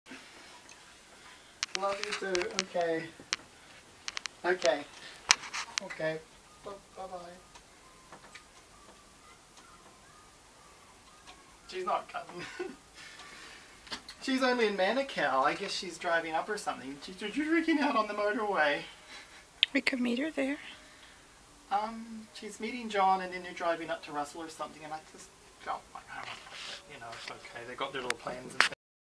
The 27 Toru St Experience of actually having a land line to ring people up from our house
The Big Bad Motorway put people off from visiting us there. Anna sold it anyway, and we had to leave. We are happier where we are, MUCH happier with nice neighbours on every side! (:D